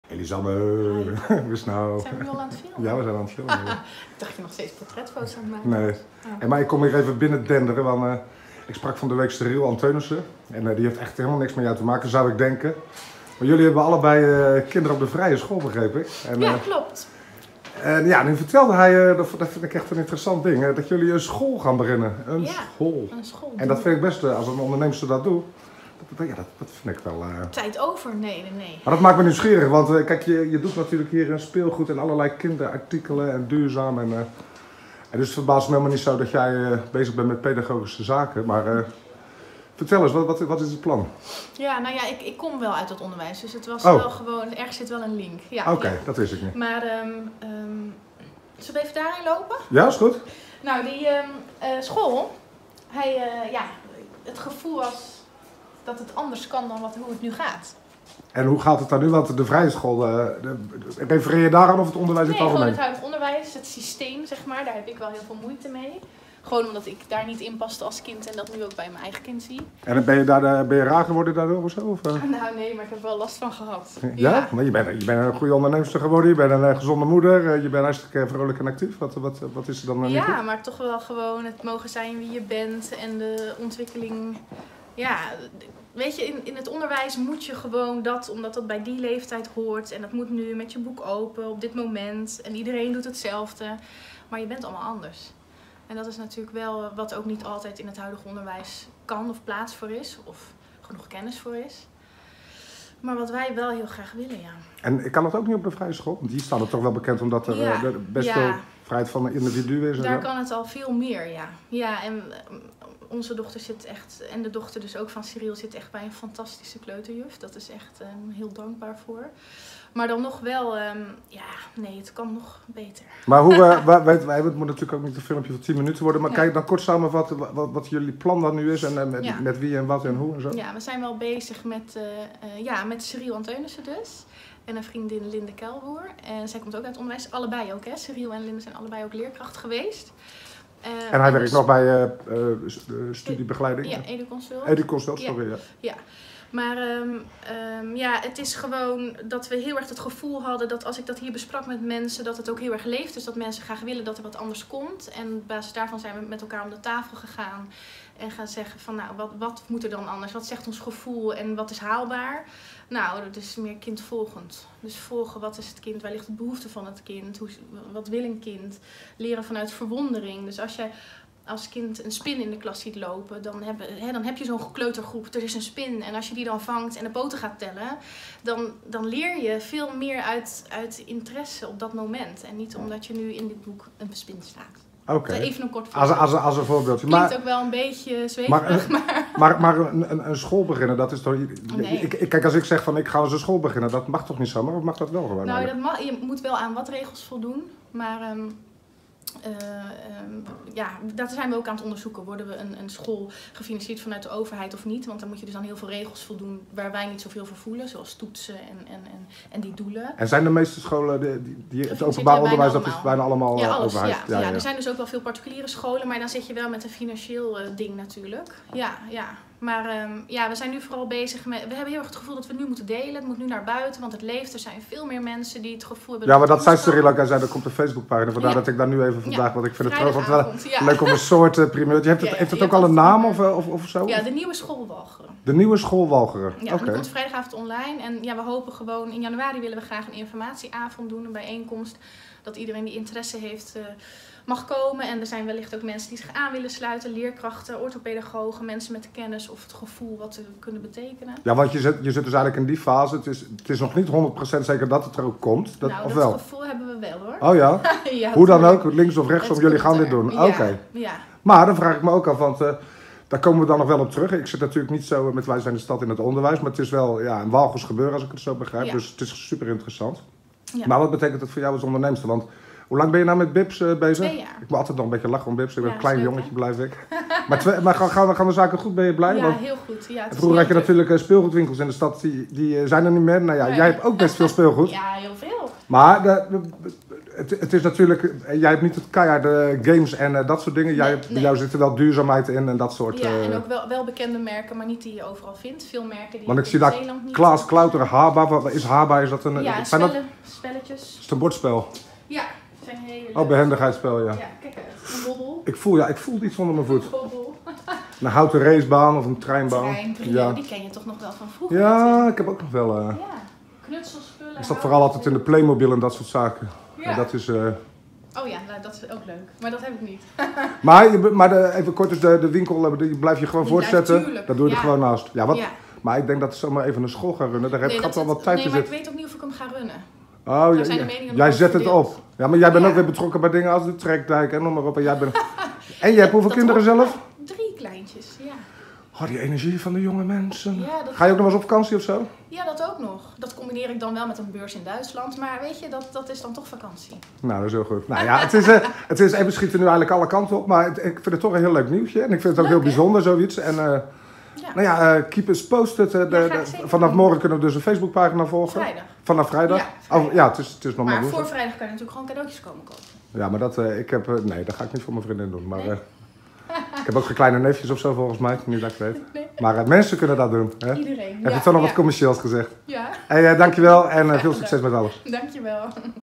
Lisanne Karman-Veenstra over de Nieuwe School Walcheren
We kijken van steeds minder dingen op, eerlijk is eerlijk, maar gisteren stonden we ook weer even te stuiteren, op Woonboulevard Middelburg 'de Mortiere'. Daar liepen we Ceriel Antheunisse tegen het lijf, bij de Kwantum. Hij was aan het speuren naar een spiegel voor in de wc van zijn gepimpte huis. Toen hadden we het vervolgens, uiteraard over allerlei bijzondere winkelgewoonten, van allerlei mensen, maar kwamen we ook te praten over het werk van Ceriel.

"We gaan binnenkort een nieuwe school beginnen."

Huh! Hallo! Wat zegt u? Een school dus. De Nieuwe School Walcheren. Ik was een en al oor, zeker toen hij vertelde dat hij dat onder meer gaat doen met Lisanne Karman-Veenstra van Bib's in de Langeviele. Hoe dan? Wat dan? "Ga maar bij Lisanne langs, die vertelt je alles!"

En vandaag waren we bij Lisanne hoor. En ze heeft het uitgelegd. Is het 't einde van Bib's? Nope! Wordt het een bijzondere school? Daar heb je een goede kans van. Maar misschien ook wel een hele goede, daar gaan we niet over. Wat we wel steeds vaker horen en lezen is dat er veel behoefte is aan onderwijs dat tussen de gewone lagere scholen en Vrije School Zeeland in zit. Dat jonge mensen daar dan ook gewoon voor gáán, hoe dan ook, vinden we ook een mooie vorm van ondernemerschap.

Vanaf vrijdag gaat de Facebookpagina van De Nieuwe School Walcheren live. Houd het in de gaten, als je dit interessant vindt!